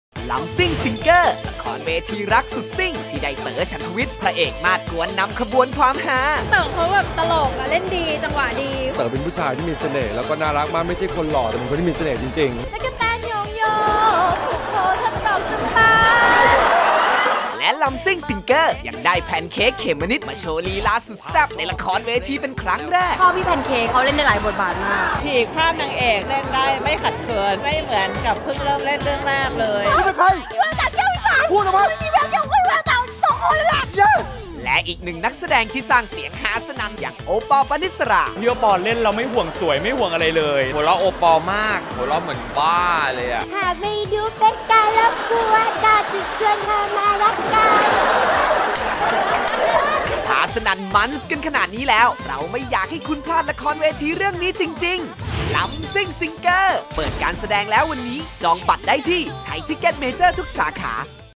ลำซิ่งซิงเกอร์ feedback 1
หลากหลายความรู้สึกของผู้ที่ได้ชมละครเวทีลำซิ่งซิงเกอร์
เปิดแสดงแล้ววันนี้ที่เมืองไทยรัชดาลัย เธียเตอร์ ซื้อบัตรแล้วที่ไทยทิคเก็ตเมเจอร์ทุกสาขา
นำแสดงโดย เต๋อ-ฉันทวิชช์ แพนเค้ก-เขมนิจ โอปอลล์-ปาณิศรา ร่วมด้วย ตู่-นันทิดา ต่อง-สาวิตรี ปุ๊กกี้-เนื้อคู่ ซันนี่ยูโฟร์